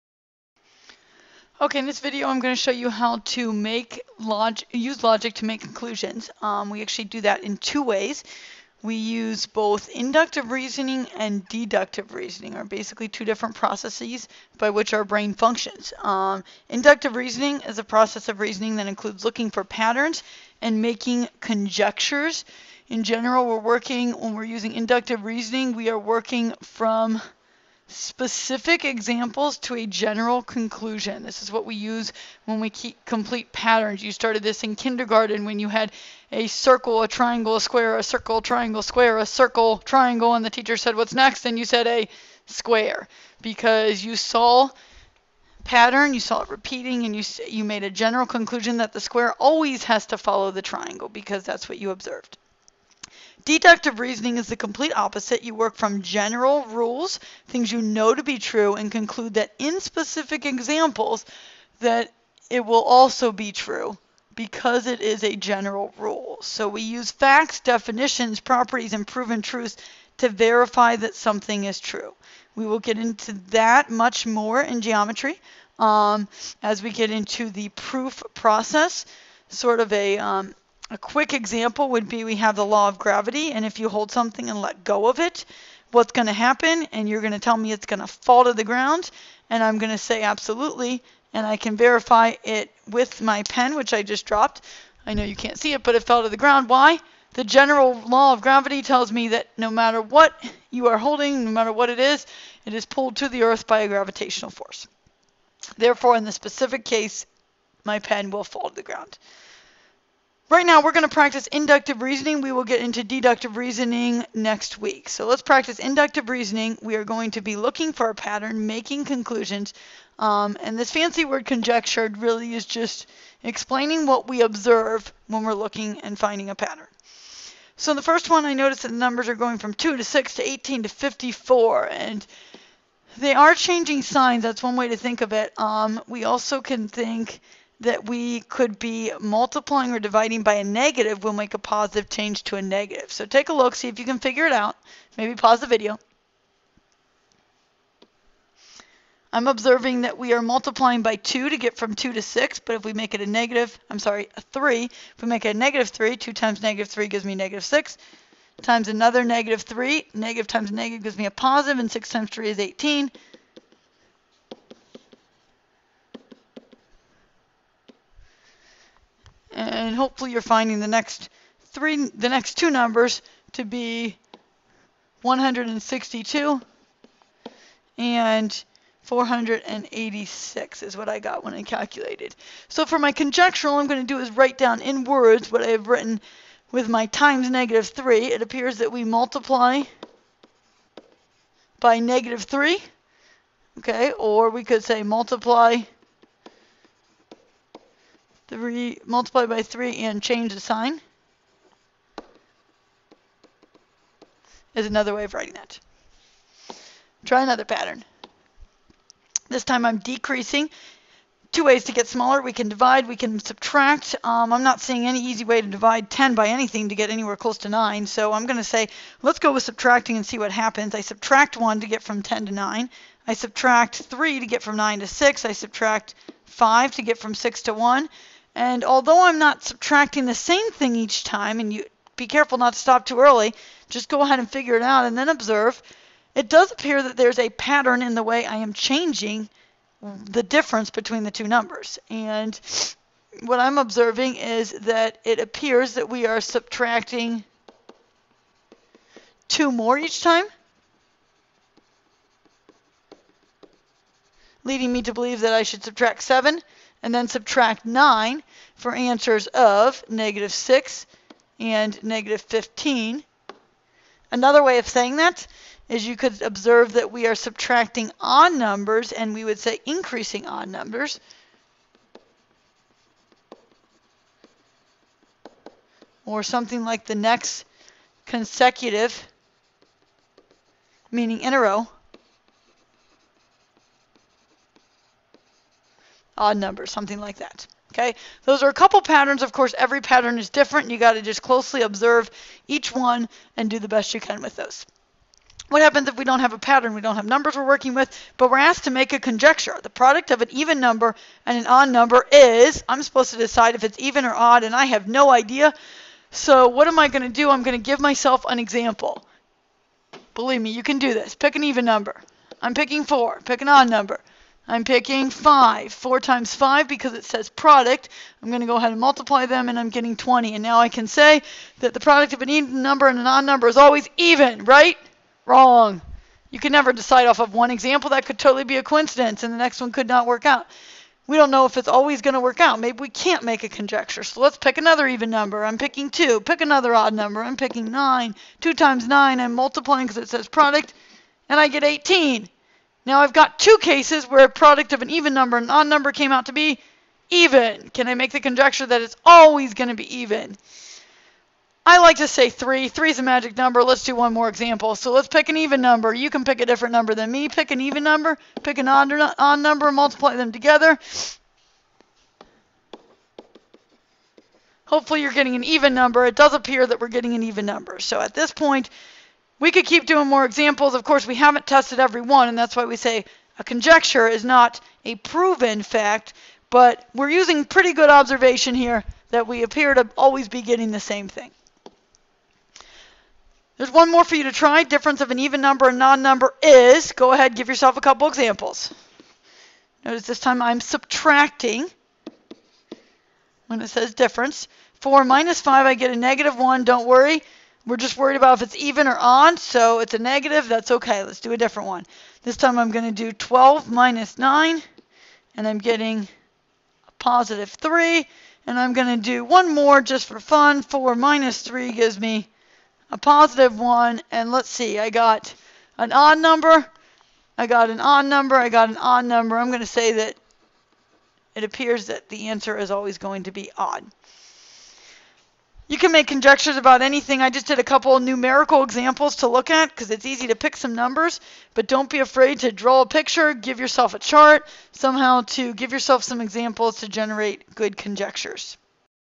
Inductive Reasoning (Section 2-1)
Learn what the difference is between inductive and deductive reasoning.  Learn how to use inductive reasoning to complete patterns and make conjectures.